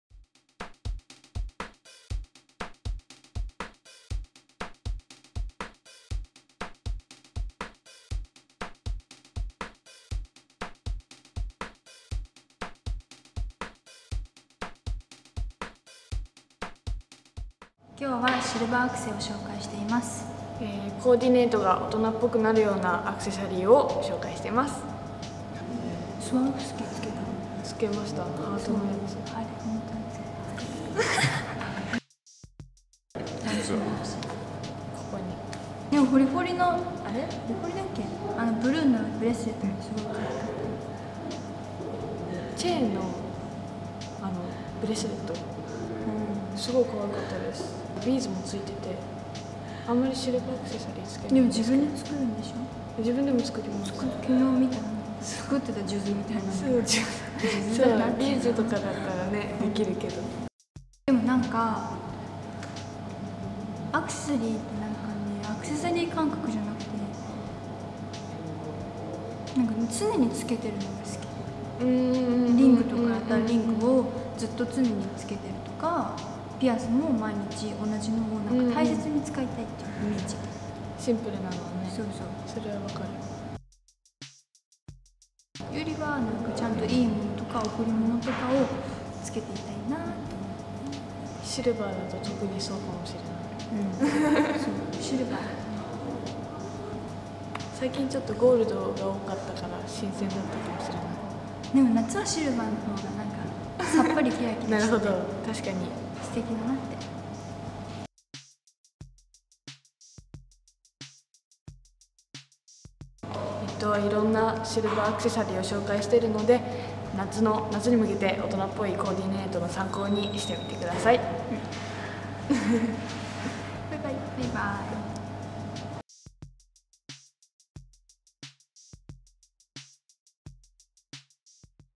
ノンノ９月号 竹富聖花・佐谷戸ミナ“仕上げはシルバーアクセ”撮影をキャッチ
ノンノ９月号“仕上げはシルバーアクセがいい感じ！”では、涼しげで華がある、大人な表情のシルバーアクセをたくさん紹介！
撮影に潜入してきたよ♪
二人が思う、理想のアクセの付け方についてもインタビュー。
この夏はシルバーアクセで、コーデを素敵に格上げしましょ！